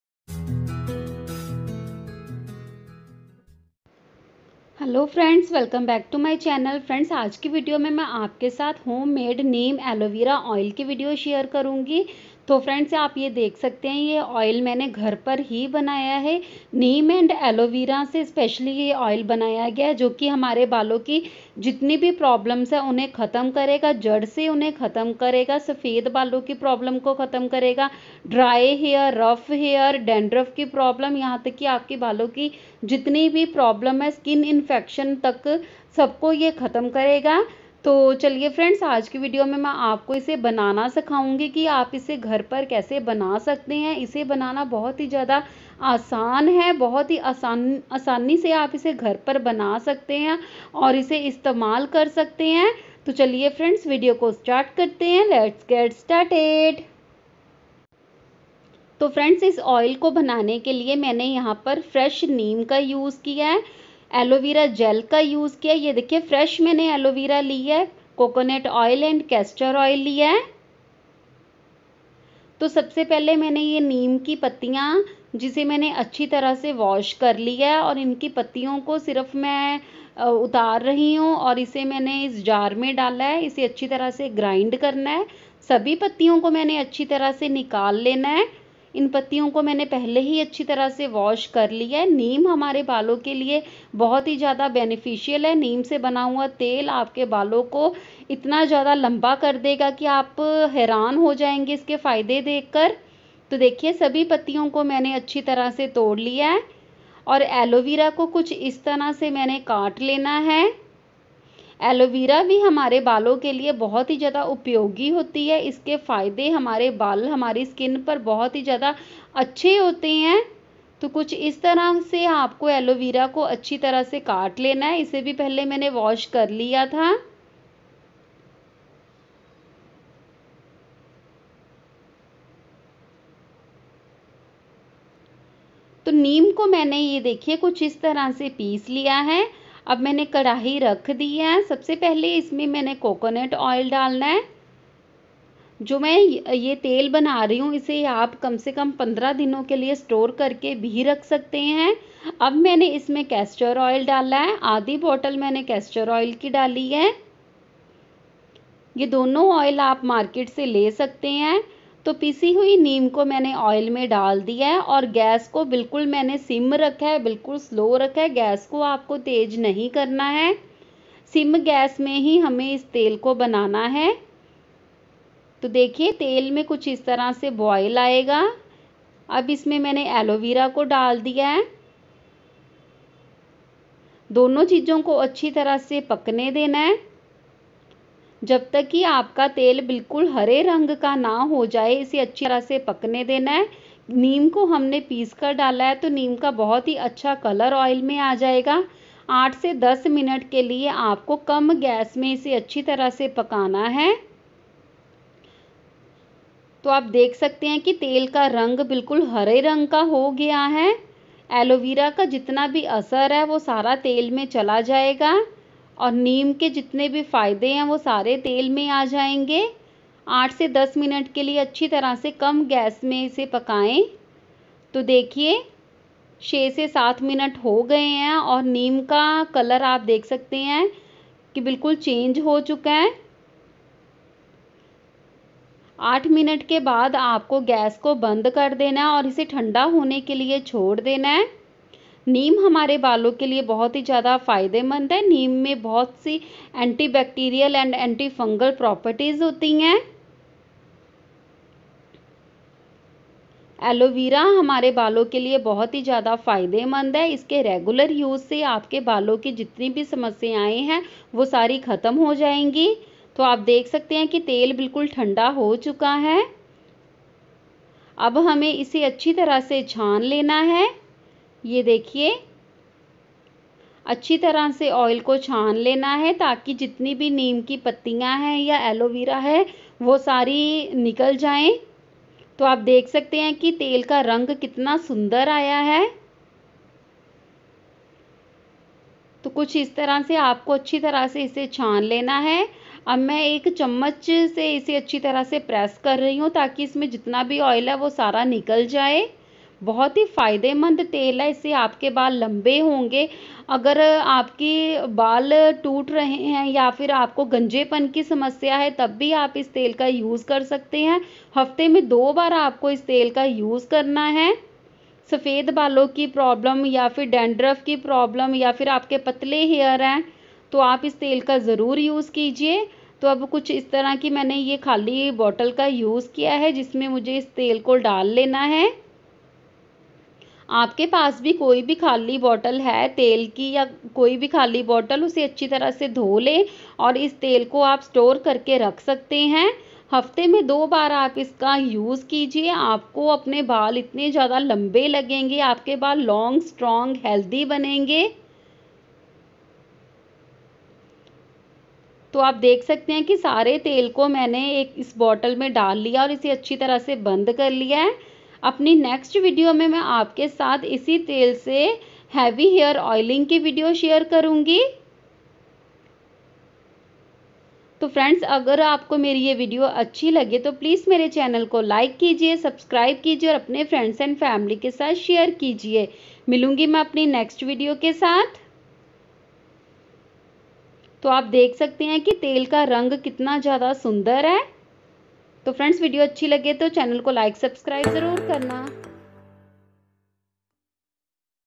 Homemade Neem & Aloe Vera Hair Oil For Double Hair Growth In Summer Session
Neem and aloe Vera oil
Homemade
Very effective
Amazing results
Summer special oil
Natural ingredients
Thanku
Friends
For
My
Have a nice day
Take care
Bye